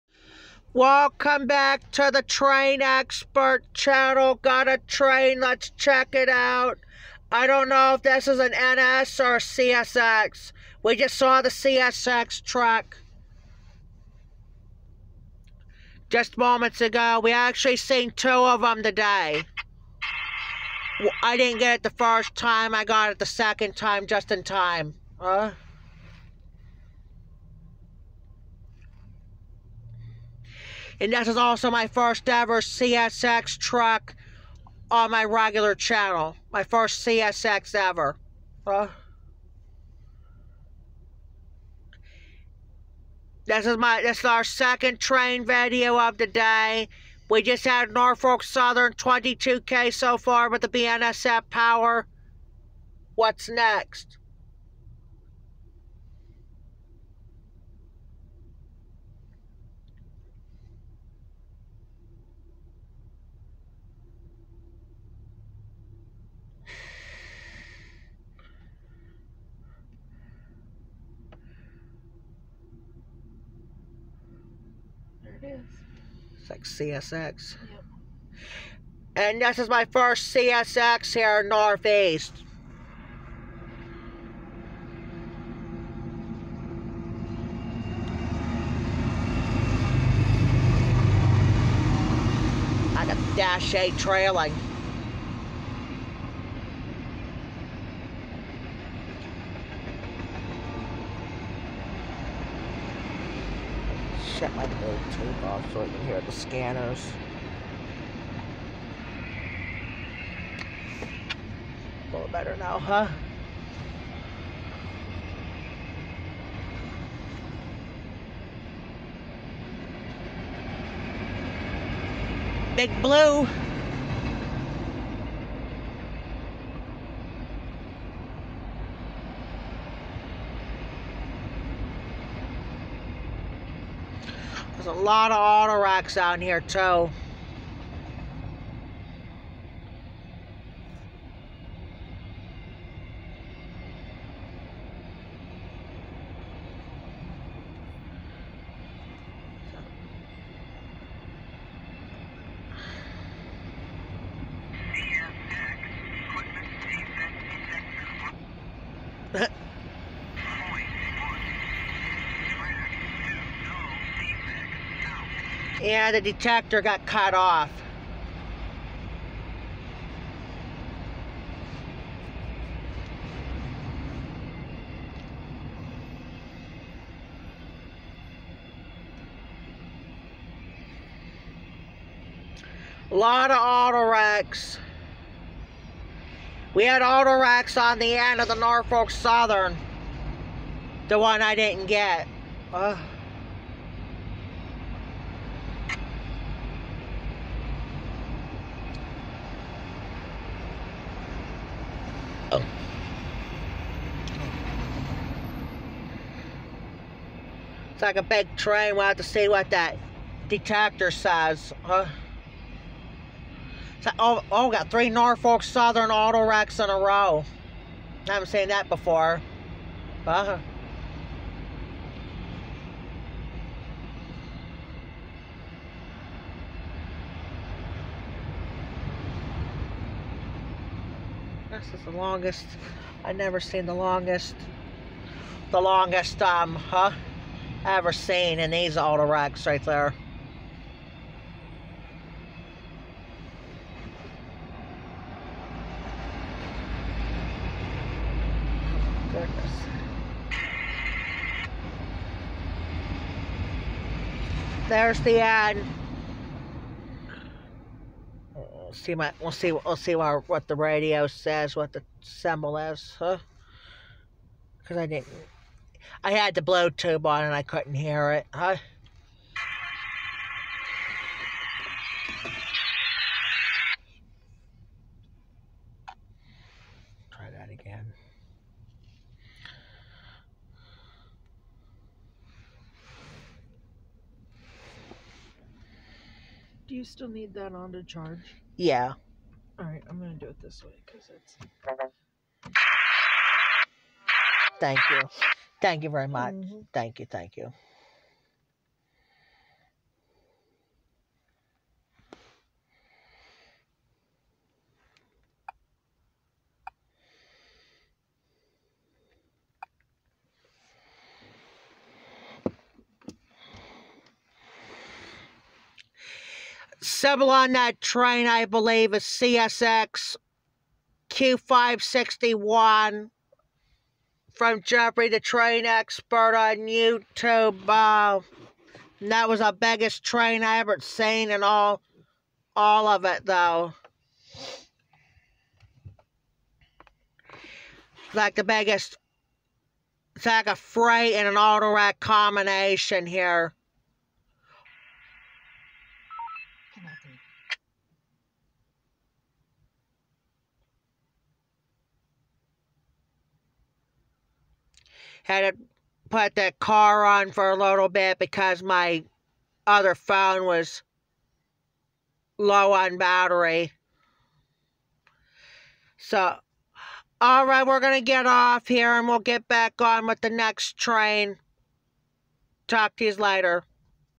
CSX Mixed Manifest Freight/Autorack Train is Heading East CSX Q561
At the Lake Short Museum and Park and Station in North East Pennsylvania